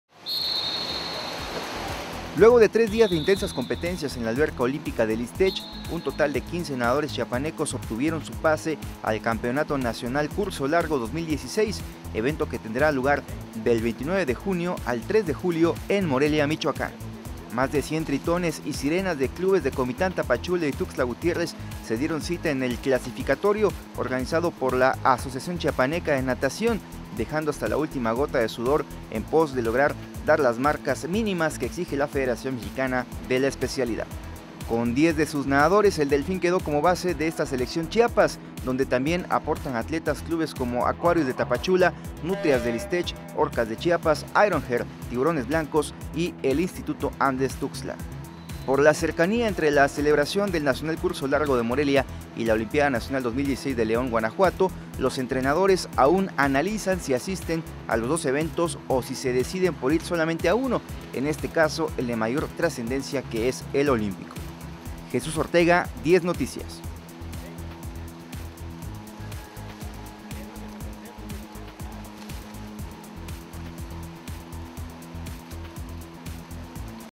15 nadadores chiapanecos clasifican al Campeonato Nacional Curso Largo 2016
Luego de tres días de intensas competencias en la alberca olímpica del Isstech, un total de 15 nadadores chiapanecos obtuvieron su pase al Campeonato Nacional Curso Largo 2016, evento que tendrá lugar del 29 de junio al 3 de julio en Morelia, Michoacán.
Más de 100 tritones y sirenas de clubes de Comitán, Tapachula y Tuxtla Gutiérrez, se dieron cita en el clasificatorio, organizado por la Asociación Chiapaneca de Natación, dejando hasta la última gota de sudor en pos de lograr dar las marcas mínimas que exige la Federación Mexicana de la especialidad.